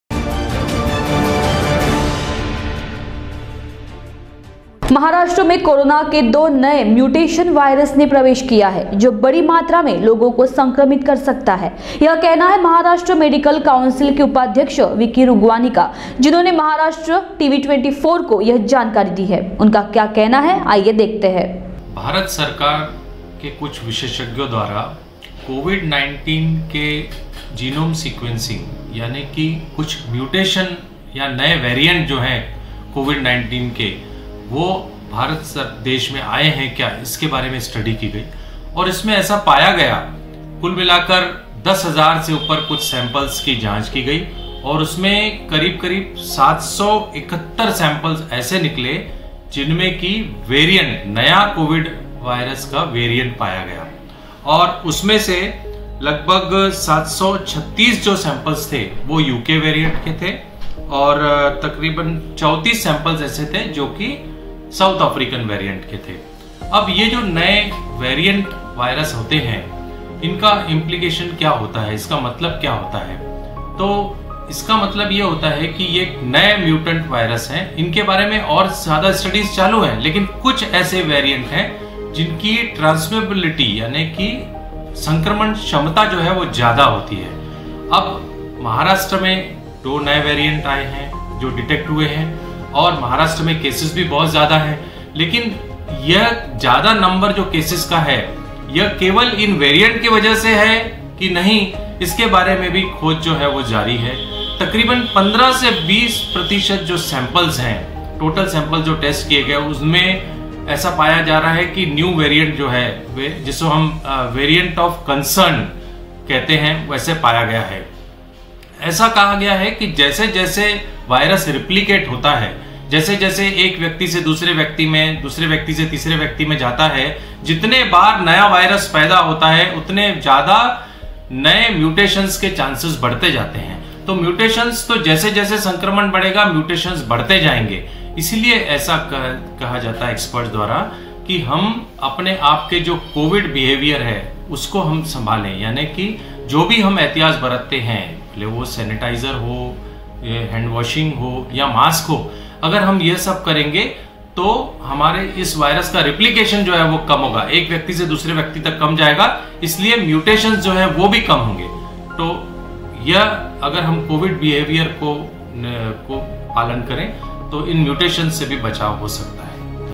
महाराष्ट्र में कोरोना के २ नये म्युटेंड ने किया प्रवेश | 25 March 2021
बड़ी मात्रा में कर सकते है संक्रमित  | 25 March 2021